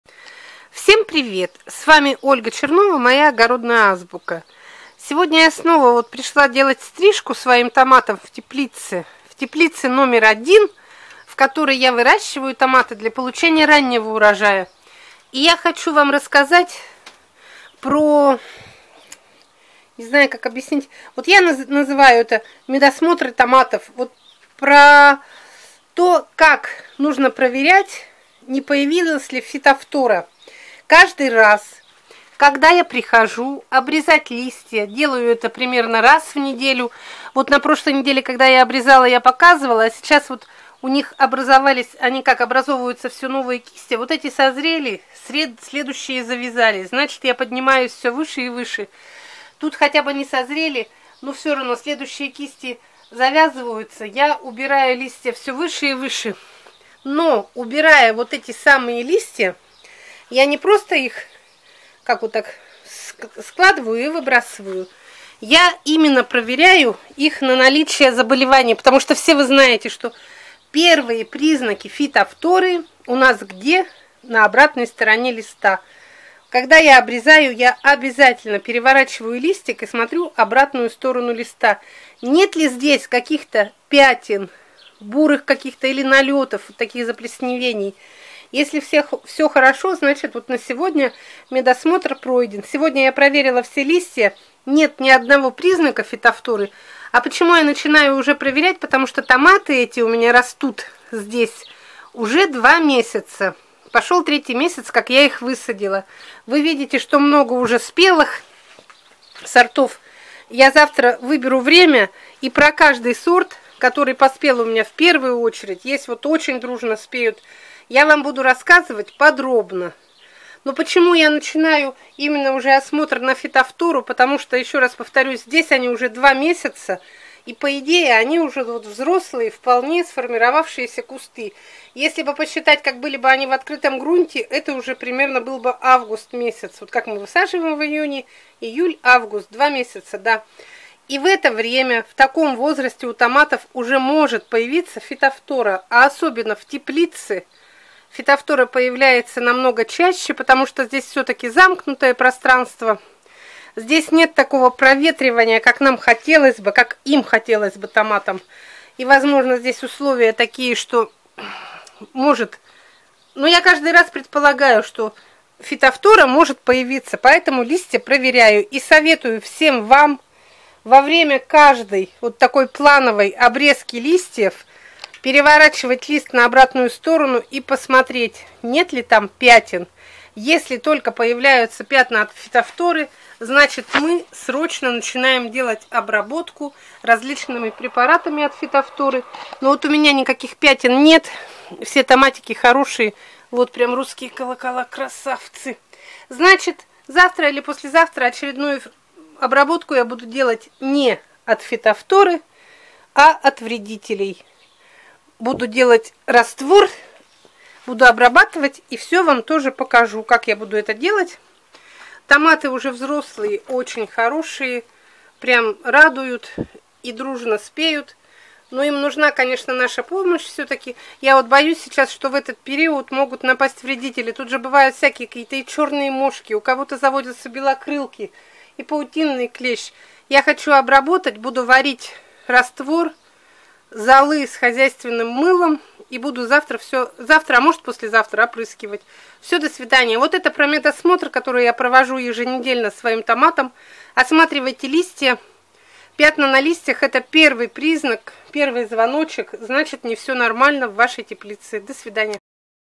МЕДОСМОТР ДЛЯ ТОМАТОВ В ТЕПЛИЦЕ. Ольга Чернова.
Всем известно, что в теплице самые благоприятные условия для возникновения заболеваний растений. Чтобы не пропустить начало развития фитофторы в теплице, необходимо не реже раза в неделю осматривать нижнюю часть листа томатов.